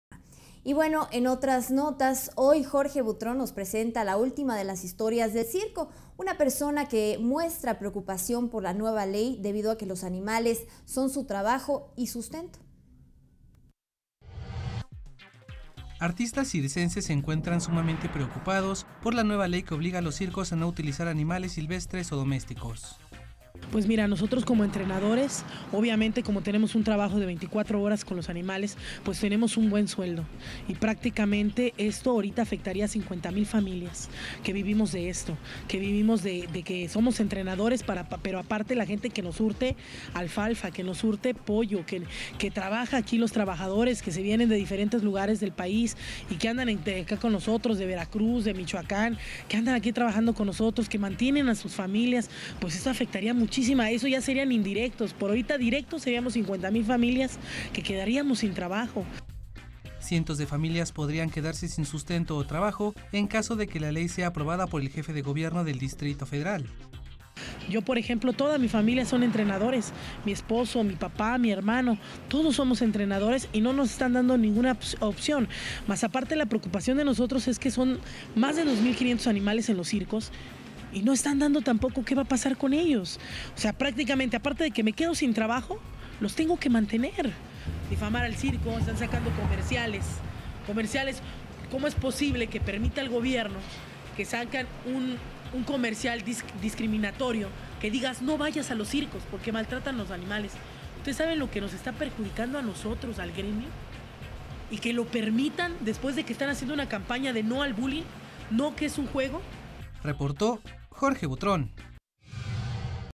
Entrenadores en circos
Cirqueros dicen que ley de CircosSinAnimales afectaría a 50 mil familias. Greentv, La Televisión Viva